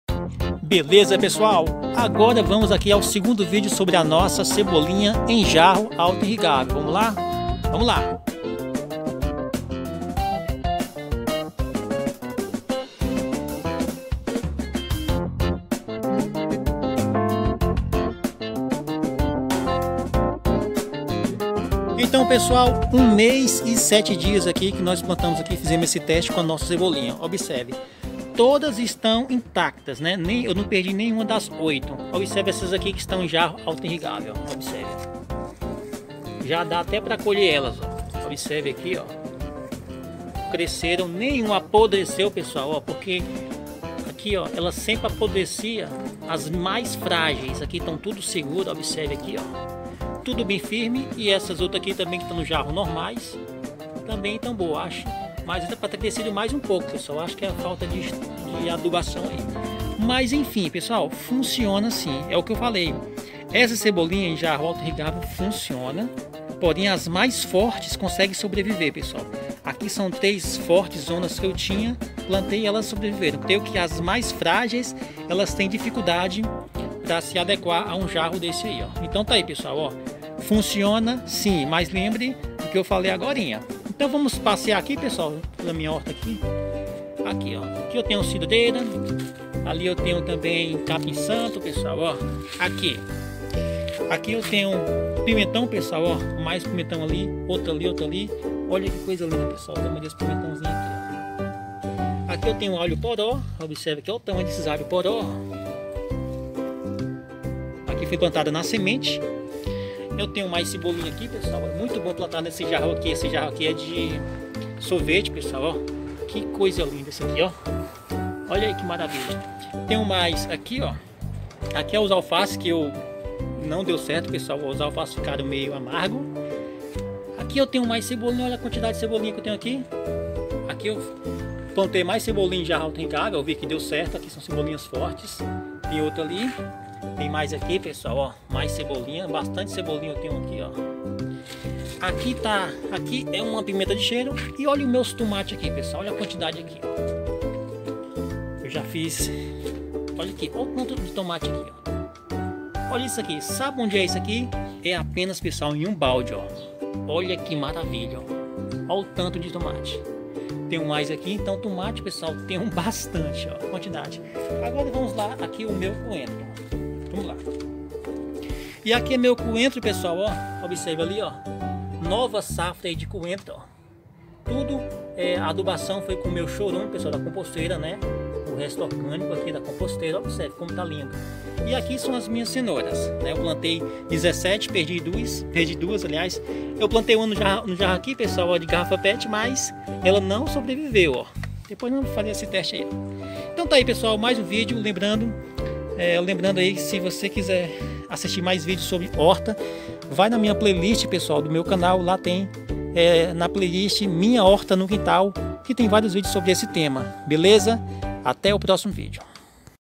Teste de cebolinha em jarro auto irrigável ( feito em garrafas pet ) ( vide 2/ 2)
Nesse vídeo fizemos um teste plantando  cebolinha   em jarro auto irrigável  feito de garrafa  pet  .. ..